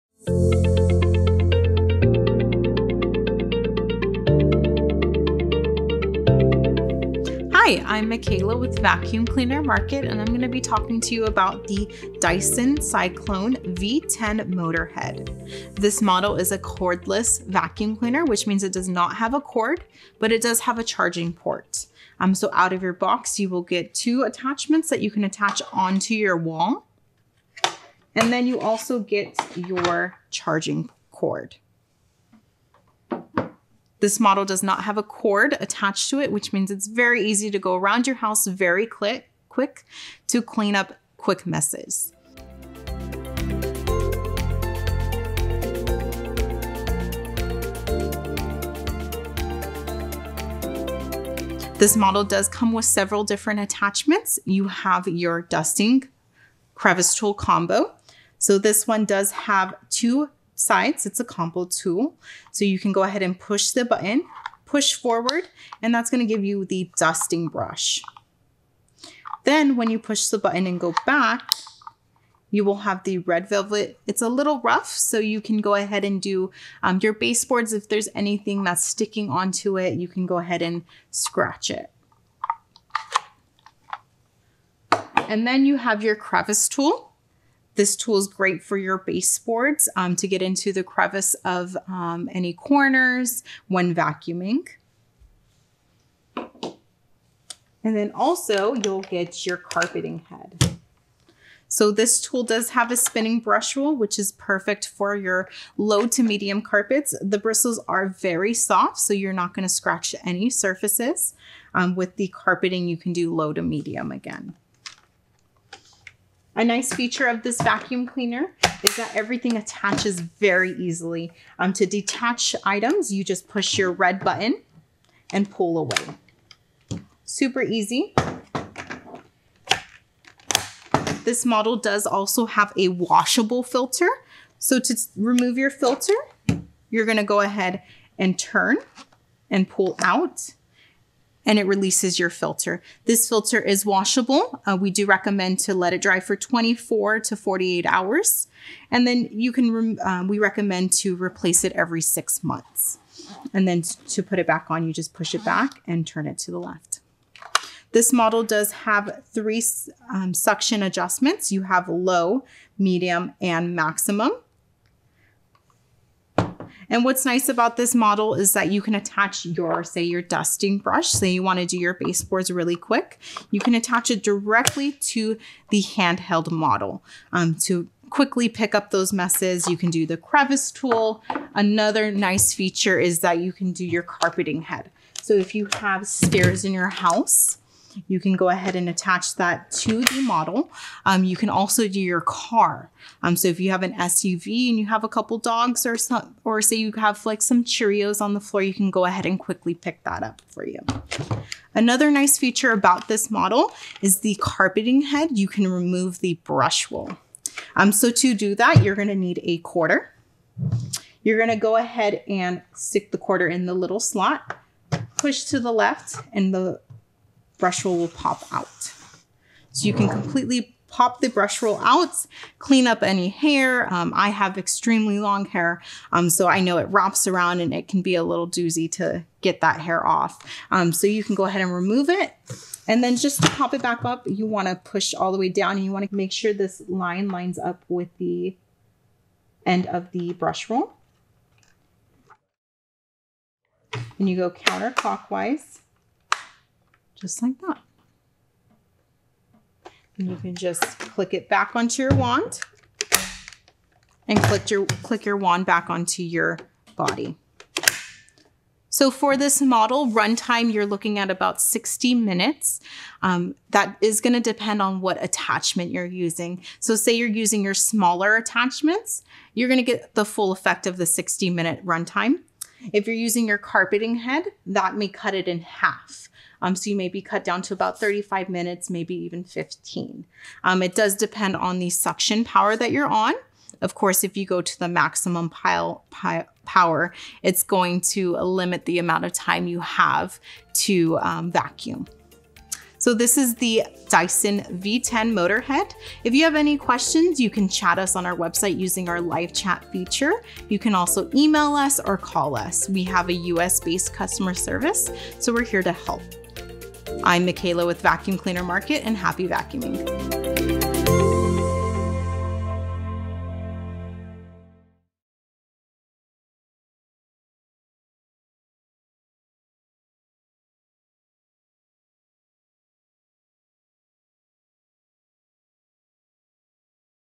What You Get Unboxing The Dyson V10 Motorhead Lightweight Cordless Stick | VacuumCleanerMarket.com
The Dyson Cyclone V10 Motorhead is a cordless, bagless, stick vacuum. This machine can be converted to a handlheld unit, for easy cleaning on stairs, automobiles, and those hard to reach areas. With a 60 minute run time (with use without motorized accessories) and (3) speeds, this machine is a powerful addition to your cleaning arsenal. The Cyclone V10 comes standard with a 2 year warranty which begins the date of purchase. This machine comes standard with (3) specialty cleaning attachments and a combination / dual -function cleaning attachment for both hard flooring and low pile carpeting. Specialty cleaning attachments can be used with the unit in either the handheld or full stick vacuum configuration. The design of this machine is low-profile and easy to clean under furniture or hard to reach areas.

This unit filters our 99.97 percent of particles 0.3 microns and larger, making it an ideal model for those with air quality concerns and allergens / asthma. This unit weighs 5.5 pounds operational weight. This is a genuine Dyson product that is new, never used.